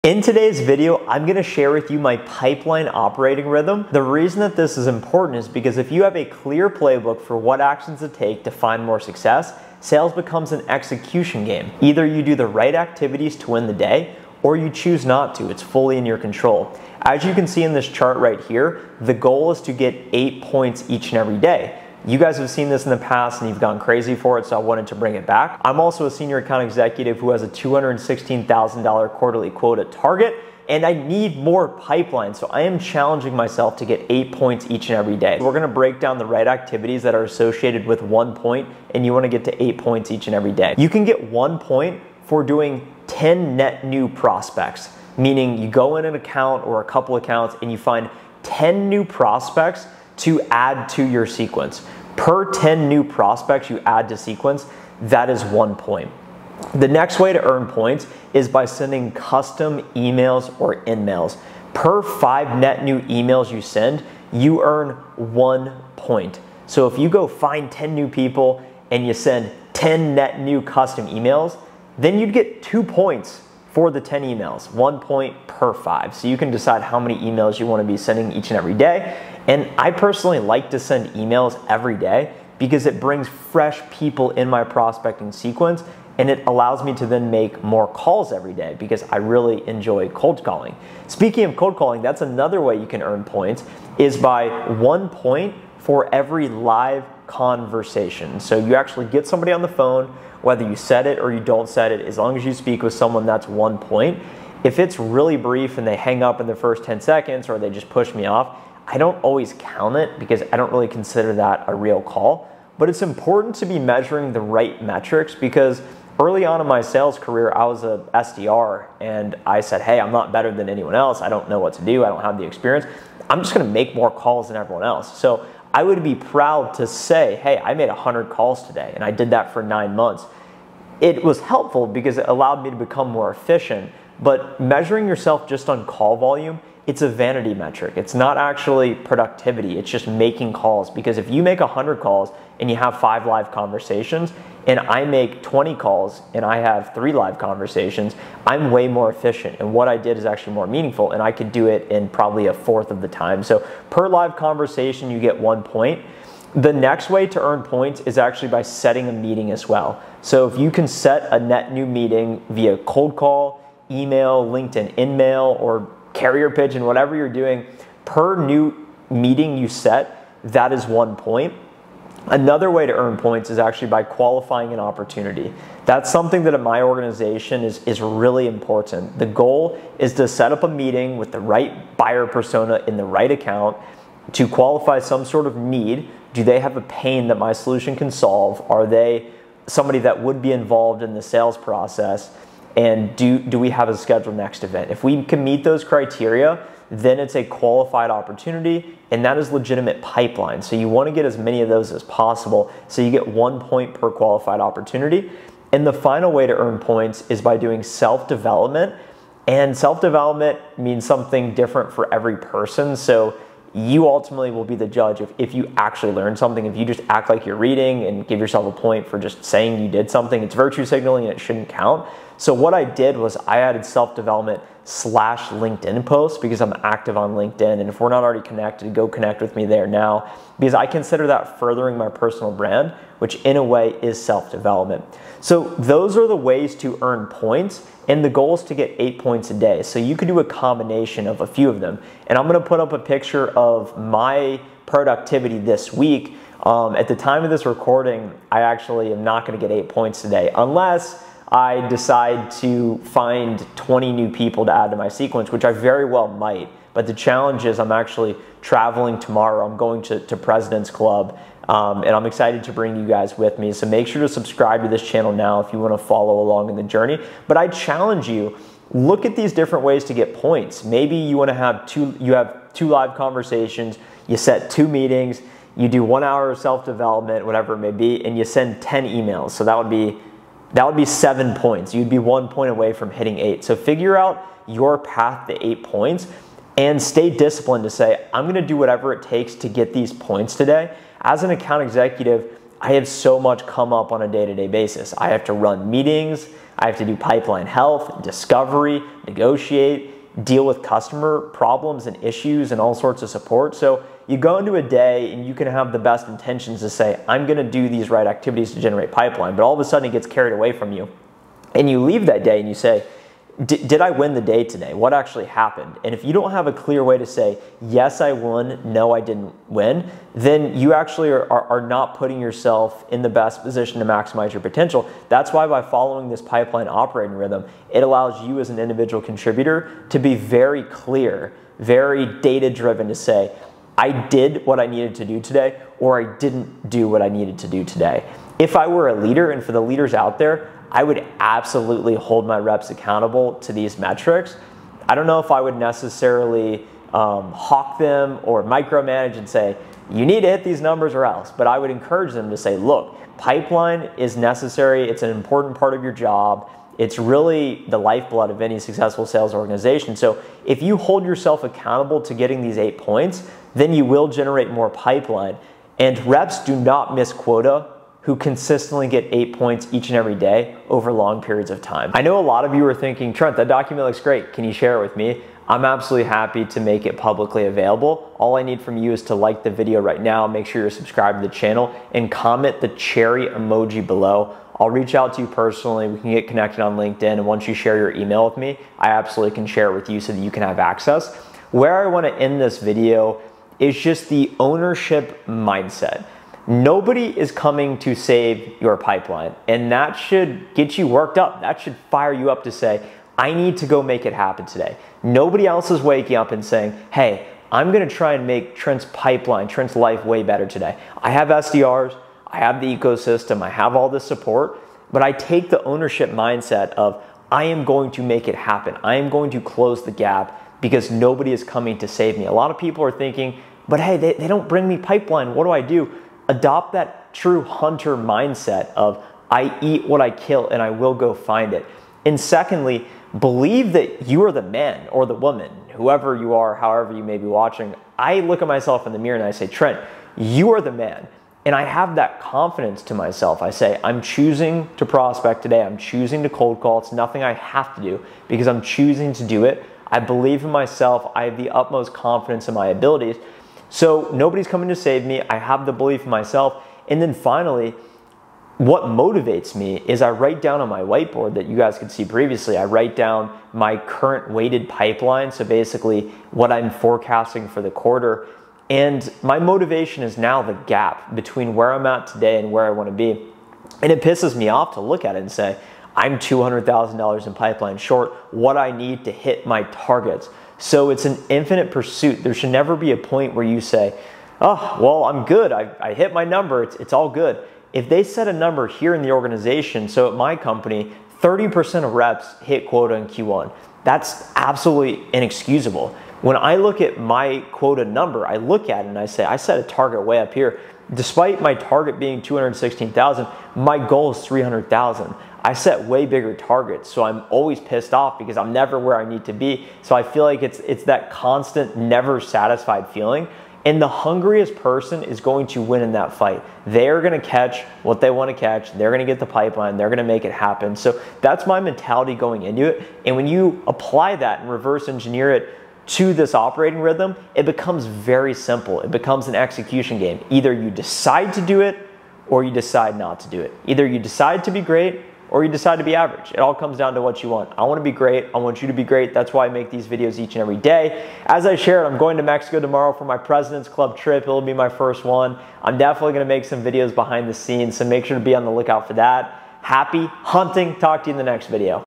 Prospecting Strategy I Use TO WIN Everyday In My Sales Job
0:00 Pipeline operating rhythm
0:35 Point system
3:55 Gamify your sales process
6:12 Mindset strategy to execute your goals